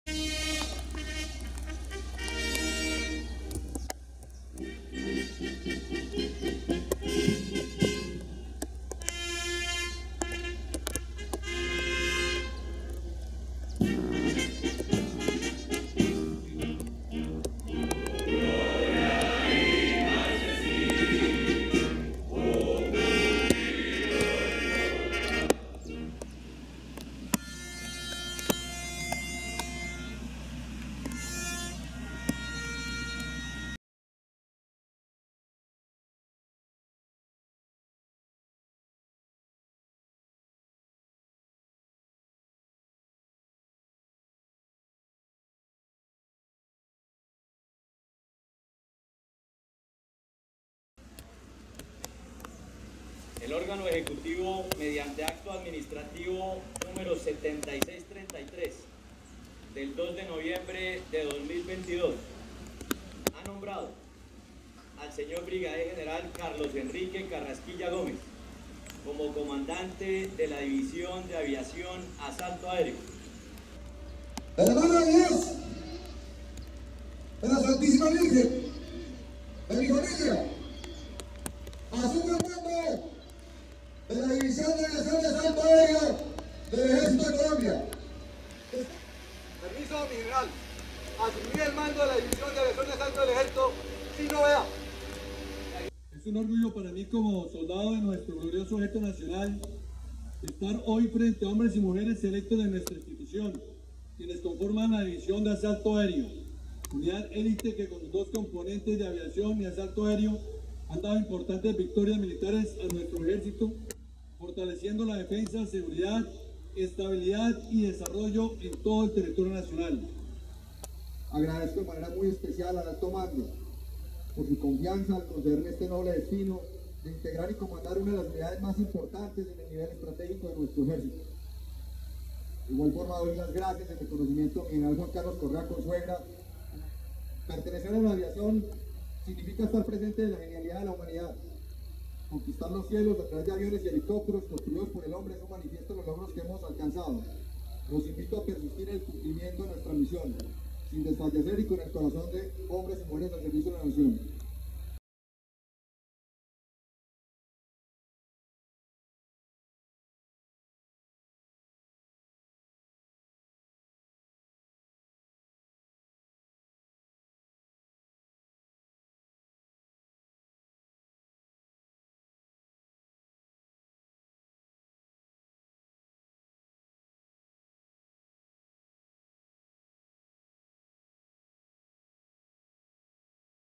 NUEVO COMANDANTE DE LA DIVISIÓN DE AVIACIÓN ASALTO AÉREO DEL EJÉRCITO NACIONAL 🇨🇴
El señor Brigadier General Carlos Carrasquilla Gómez, es quien por designación del Ejército Nacional, asume desde hoy el mando de la División de Aviación Asalto Aéreo, unidad que concentra las aeronaves de ala fija y rotatoria del Ejército Nacional y lidera las operaciones aéreas en apoyo a las operaciones terrestres brindando movilidad y maniobra de la Fuerza en todo el territorio nacional

En ceremonia militar, llevada a cabo en el campo aéreo de Tolemaida, y presidida por el señor comandante del Ejército Nacional, se llevó a cabo la transmisión de mando de la División de Asalto Aéreo, que, por cerca de un año fue comandada por el señor mayor general Juan Carlos Correa Consuegra y por el señor brigadier general Walther Adrián Giraldo Jiménez, este último como comandante encargado, apenas se dio el nombramiento del señor mayor general Correa como Inspector General del Ejército Nacional.

Hoy, asume el mando de esta unidad militar el señor brigadier general Carlos Carrasquilla Gómez, oficial de insignia del arma de infantería, oriundo de la ciudad de Medellín – Antioquia, y con cerca de 30 años de experiencia militar, luego de ser comandante de la Fuerza de Tarea Quirón en Arauca, y de la Brigada 28 en Vichada.

El nuevo comandante de la División de Aviación Asalto Aéreo, ha realizado diferentes estudios profesionales y militares tanto en el país, como en el exterior, lo que le permitirá tener un amplio conocimiento para conducir operaciones de aviación y operaciones especiales contra el narcotráfico y la explotación ilícita de yacimientos mineros eficientes y seguras, brindando apoyo oportuno y permanente a las unidades territoriales y a la población civil, aprovechando la movilidad y maniobra que brindan las aeronaves de ala fija y rotatoria, así como su capacidad de llegar a los lugares más remotos del país.

En su discurso, el nuevo comandante recalcó la importancia de continuar trabajando por la seguridad y defensa del país, además agradeció a los señores generales Correa y Giraldo por su gran labor en esta unidad militar en pro del bienestar de los colombianos, labor que continuará fortaleciendo en aras de que las aeronaves del Ejército sigan siendo las alas en las botas de los soldados, y el apoyo incondicional para los colombianos.